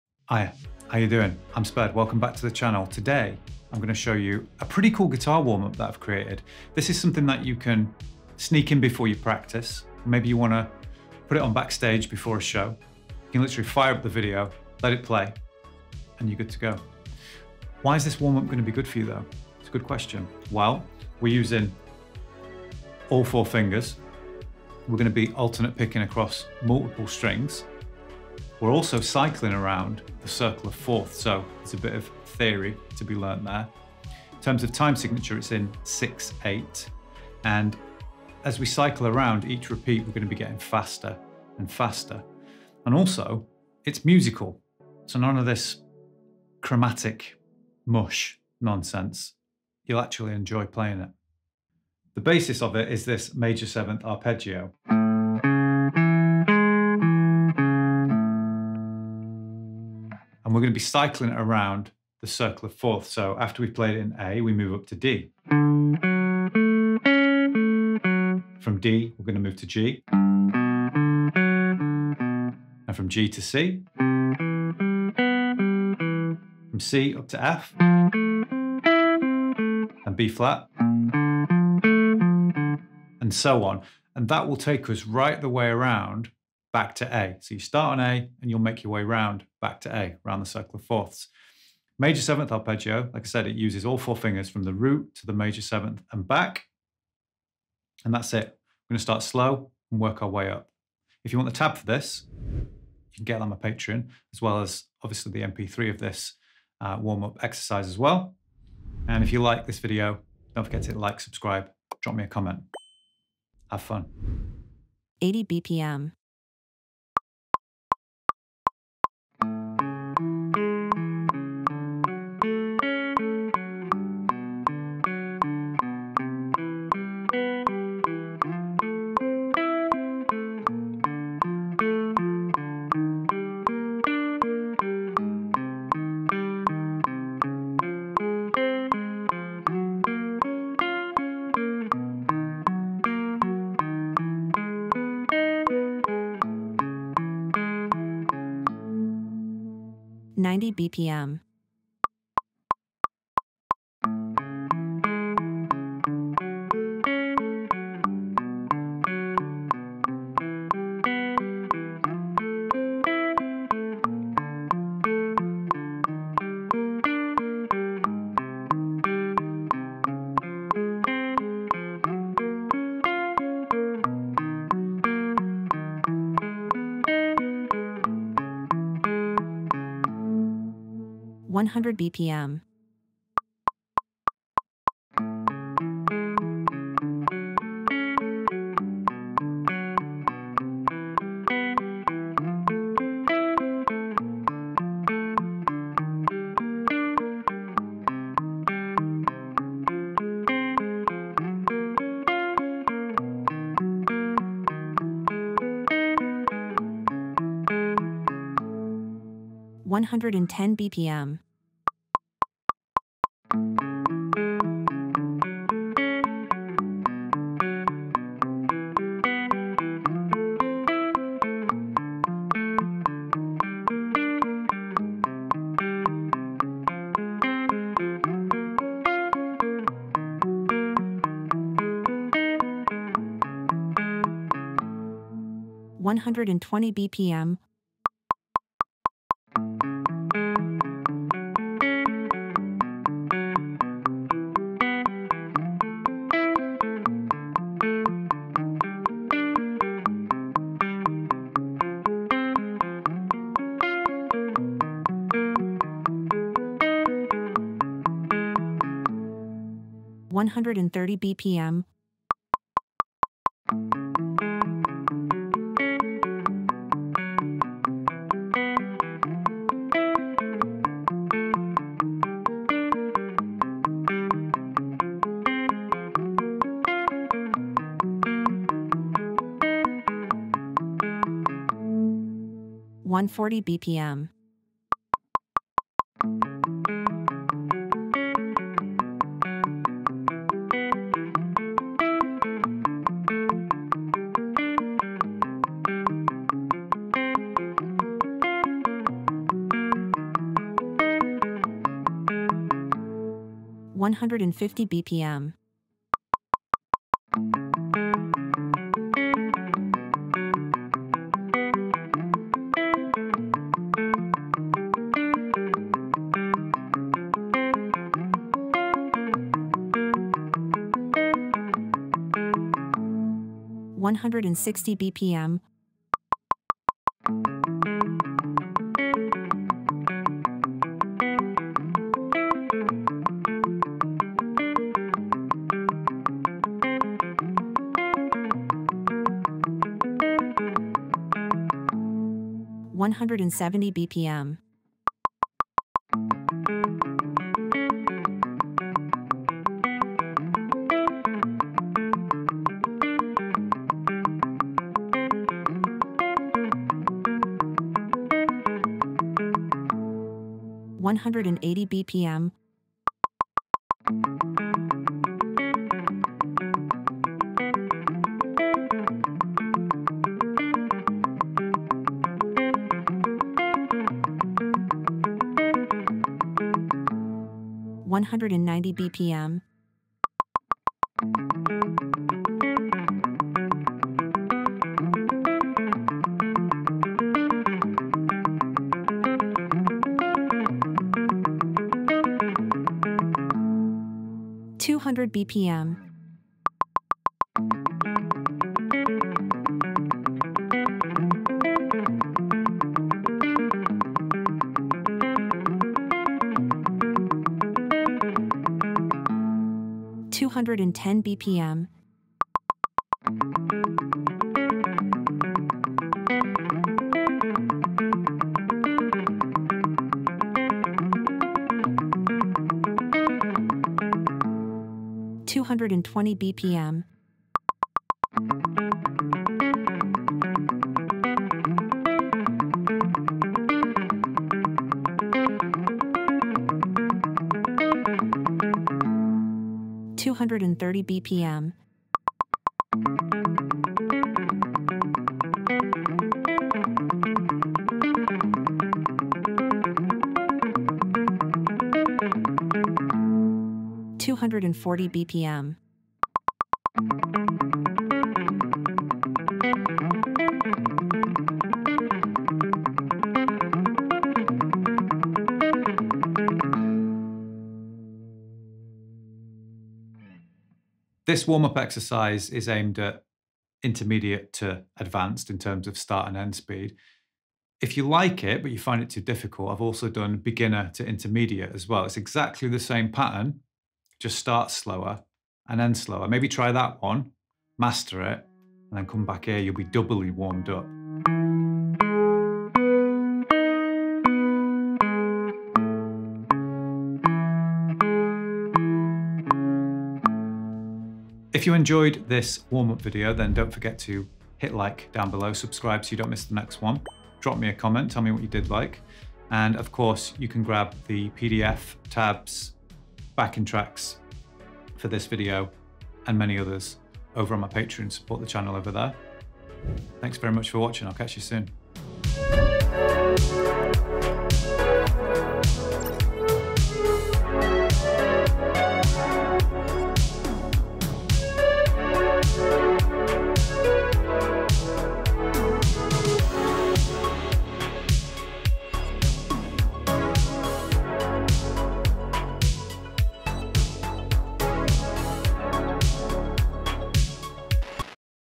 6 minute GUITAR WARM-UP: Intermediate to Advanced
This 6 minute warm-up is designed to get your fingers cooking as well as the brain. The basis is a Major 7th arpeggio that cycles around a circle of fourths A through to A.

If you would like the isolated backing track without me playing, or the synth behind as well as a fully annotated PDF and guitar tab you can grab that on my Patreon linked above.

0:00 - Why is this warm-up good?
0:51 - What you need to know
1:54 - 6 Minute Guitar Warm-Up
7:51 - Final Thoughts